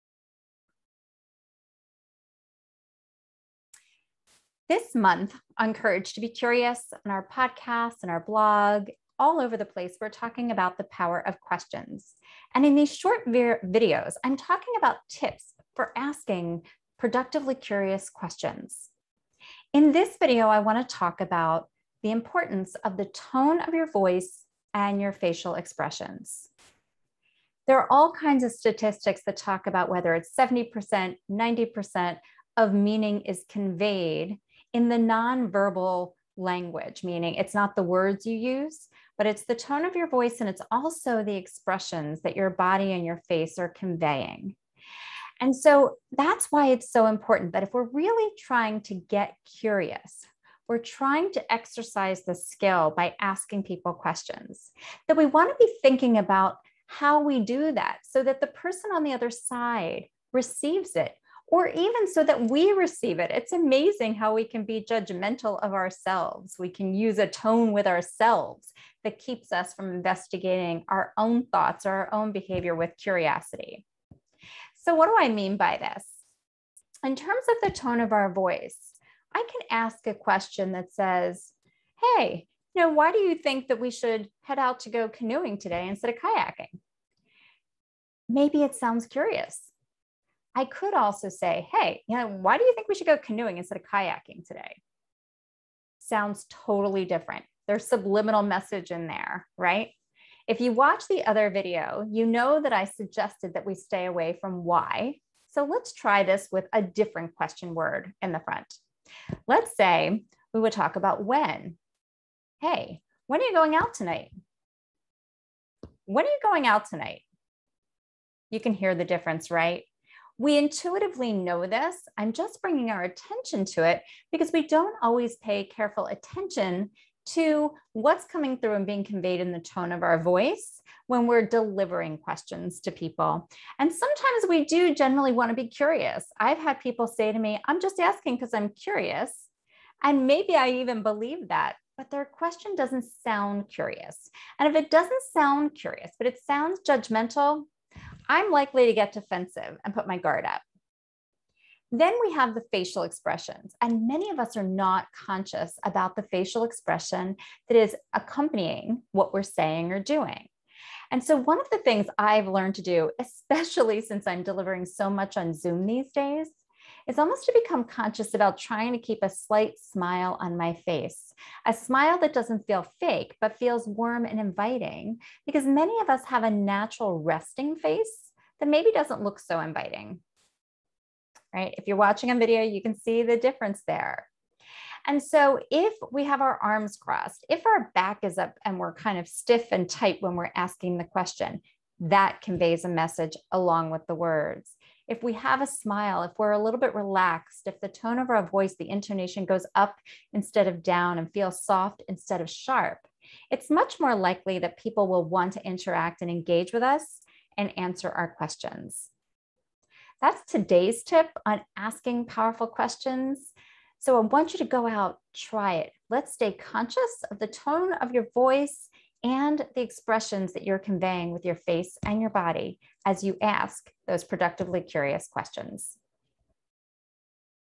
Does Your Expression Match Your Question?
In this video, Adina discusses why the tone of your voice and your facial expressions matter when you're asking someone questions.

Follow all our other platforms!